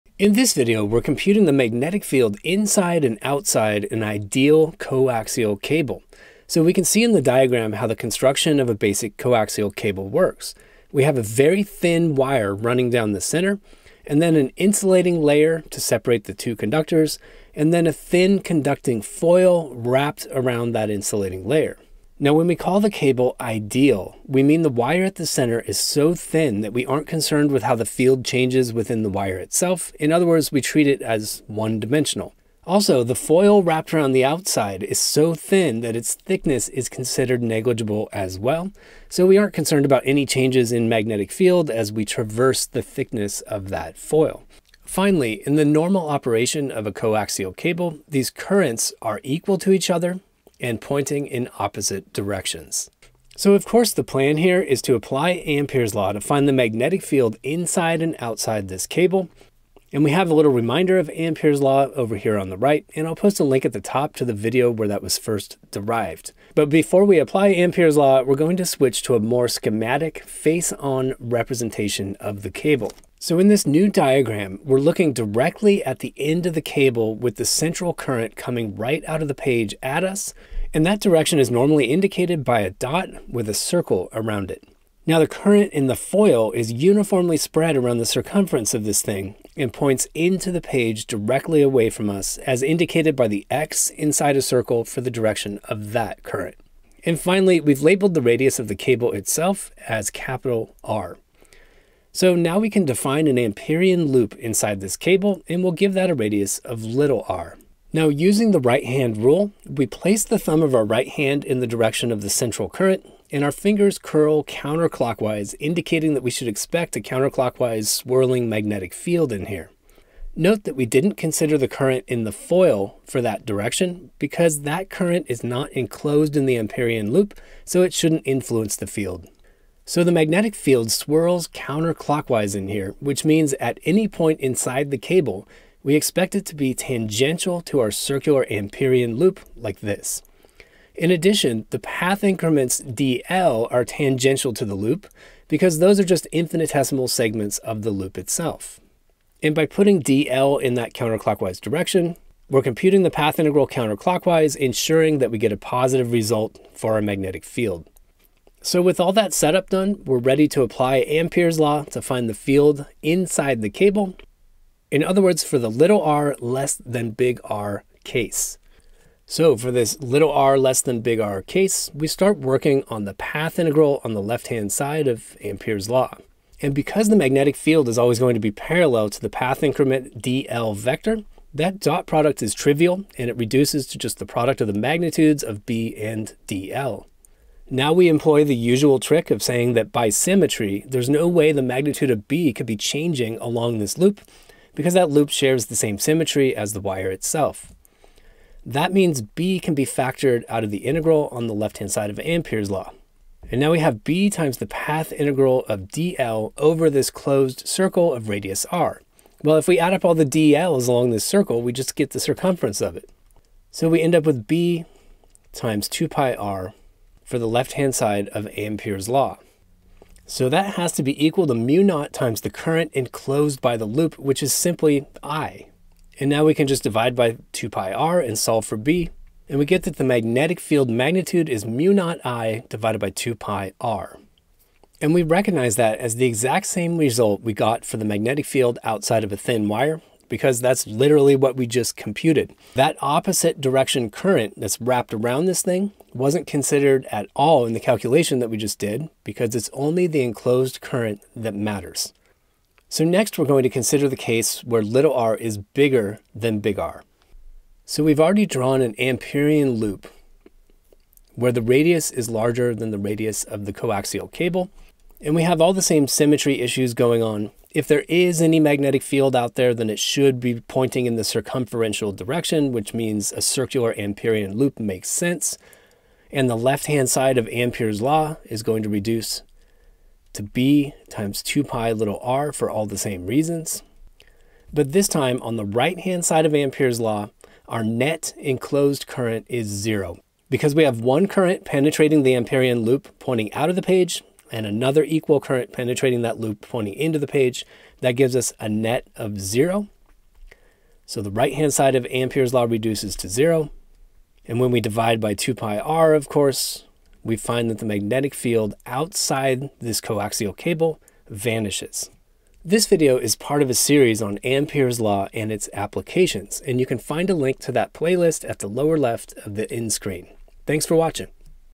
Find the magnetic field of a coaxial cable using Ampere's Law.
In this video, we calculate the magnetic field inside and outside an ideal coaxial cable by using Ampere's law.

We begin with a quick discussion of the construction of a coaxial cable:  a thin wire runs down the middle of the cable, then a layer of insulating material separates the two conductors.  Finally, a thin conducting foil is wrapped around the insulating layer.  During the normal operation of coaxial cable, currents in the two conductors will be equal in magnitude but opposite in direction.

Next, we show a more schematic view of the cable, viewing the end of the cable face-on.  The current in the central conductor is coming out at us, and the current in the foil layer is going directly away from us.

Now to find the magnetic field of a coaxial cable using Ampere's law, we need to define an Amperian loop inside the cable.  We use the right hand rule to argue that the magnetic field is circumferential and clockwise in this region, and we set up the direction of the path integral (defining the direction of the dl's) so that B is always parallel to dl.

Working on the Ampere's Law path integral, we use the fact that B is parallel to dl to conclude that  the dot product reduces to the product of magnitudes of B and dl.  Next, B must be constant by symmetry, so it can be factored out of the integral, and finally, the path integral of dl over the closed loop is just equal to the circumference, so the left side of Ampere's Law reduces to simply B*(2pir).  On the right side of Ampere's law, we have mu_0*I_enc, but the enclosed current is just I.  Solving for B, we get the classic result B=mu_0*I/2pi*r, which is the same result we obtained for a simple wire!

Finally, we compute the magnetic field outside the coaxial cable.  We set up an Amperian loop enclosing the entire cable, and the left side of Ampere's law simplifies in exactly the same way as before.  This time, the NET enclosed current is zero, since the two currents penetrate the Amperian loop in opposite directions.  So . . . the magnetic field outside a coaxial cable is zero, and we're done!